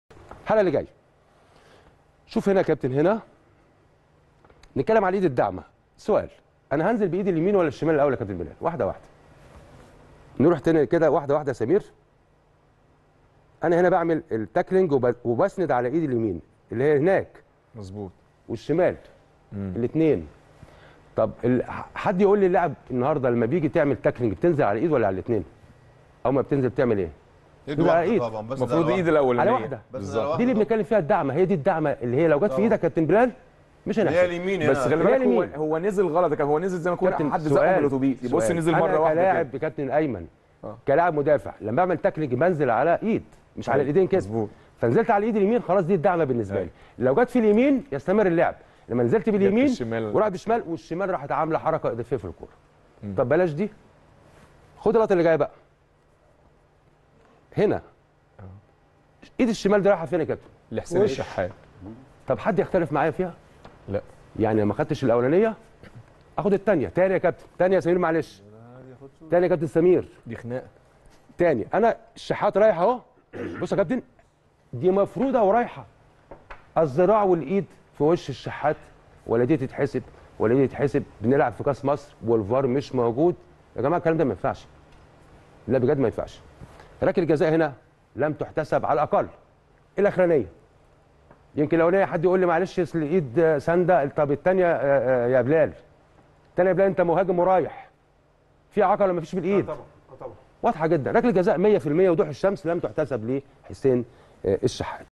الخبير التحكيمي ناصر عباسركلة جزاء صحيحه لم تحتسب لصالح حسين الشحات واضحة وضوح الشمس
الخبير التحكيمي ناصر عباس
ركلة جزاء صحيحه لم تحتسب لصالح حسين الشحات واضحة وضوح الشمس | الأستوديو التحليلي